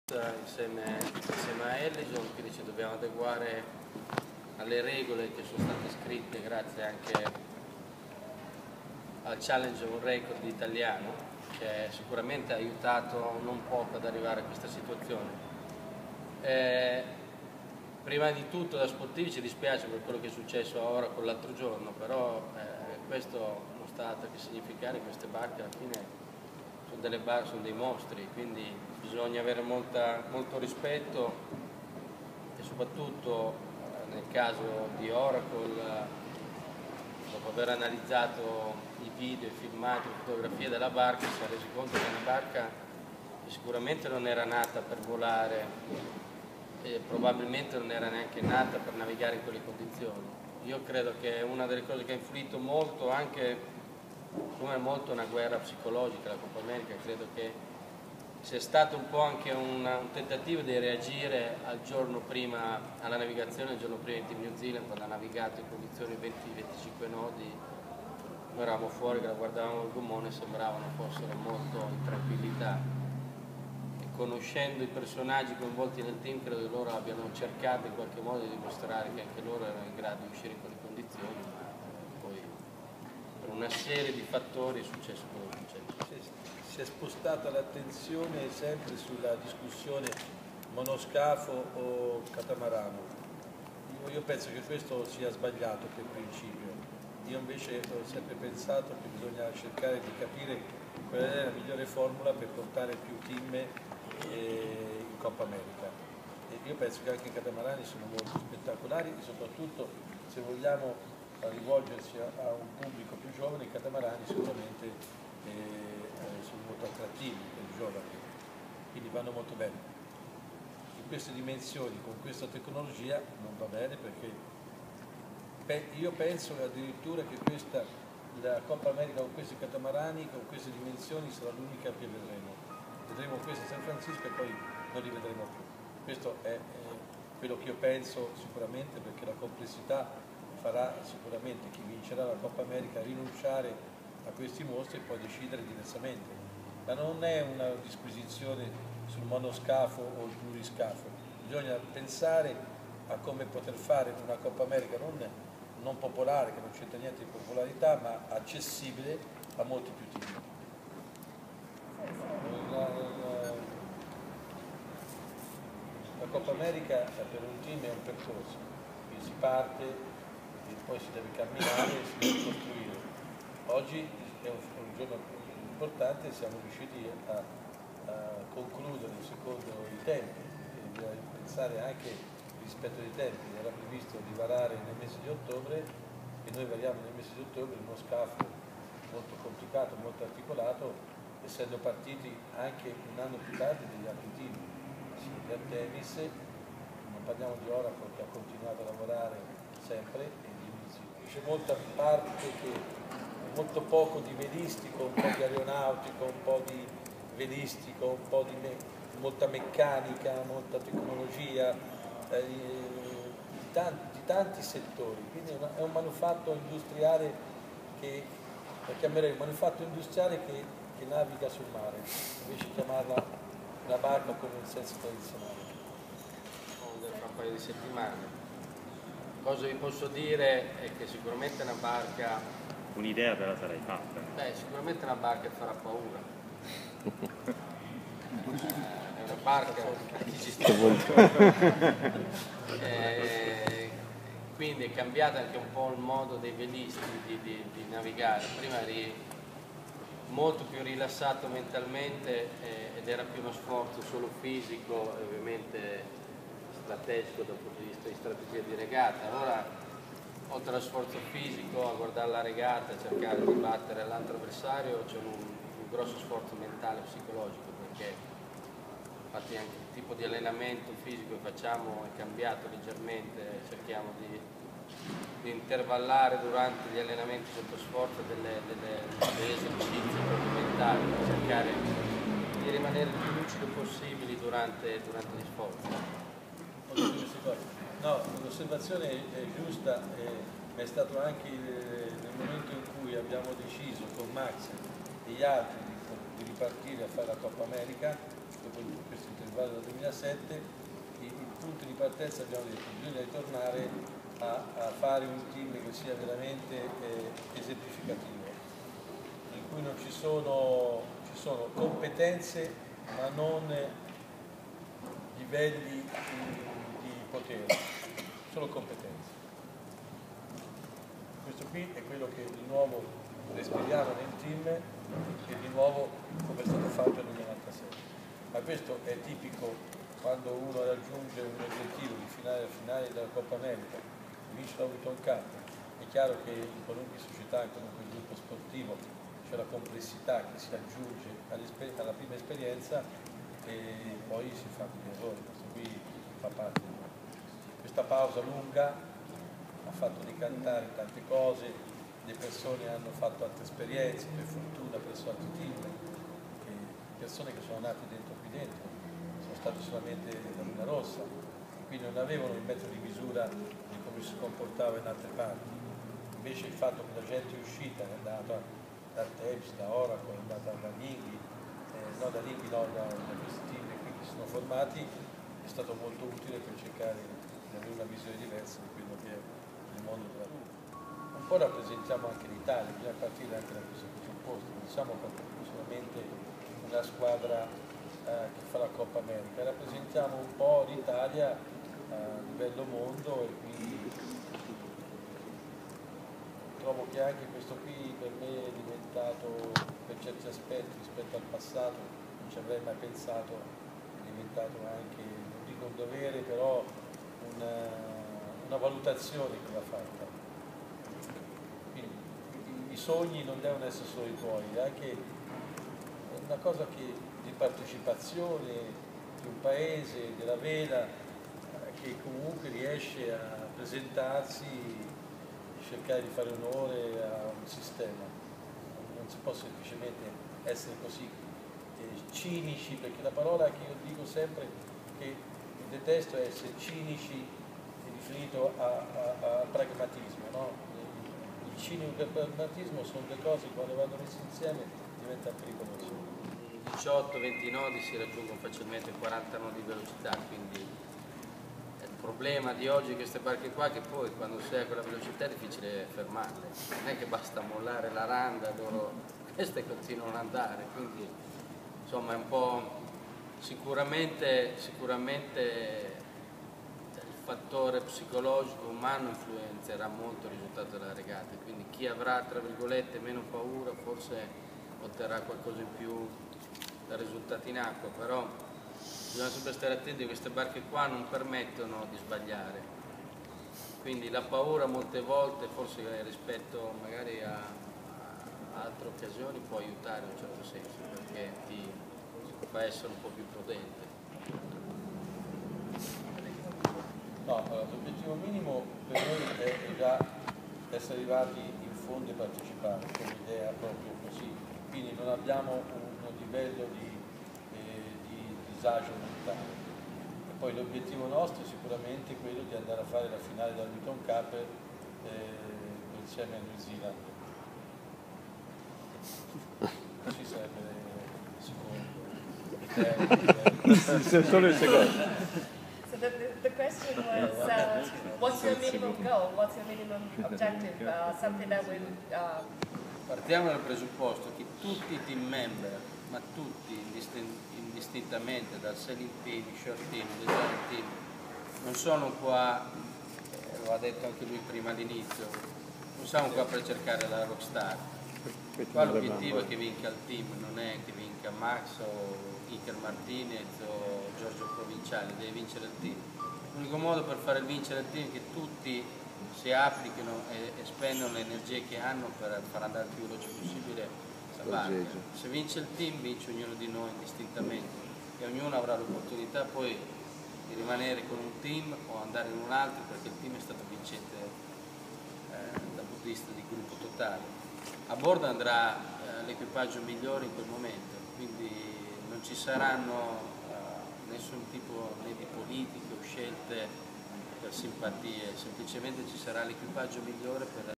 Luna Rossa launch, Patrizio Bertelli e Max Sirena press conference
Auckland, 26 ottobre 2012